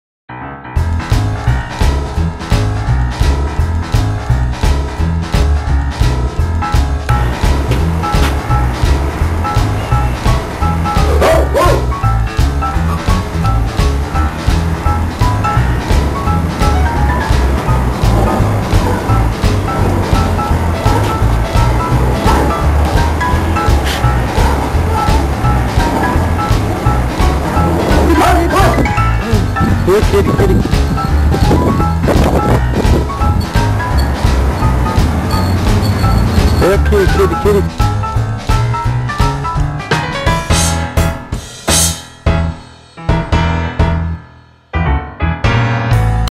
Bear
Alaskan Malamute
They are independent, can be stubborn and are a generally large breed of domestic dog, bred for use as a utilitarian dog and later an Alaskan sled dog and loyal to their owners.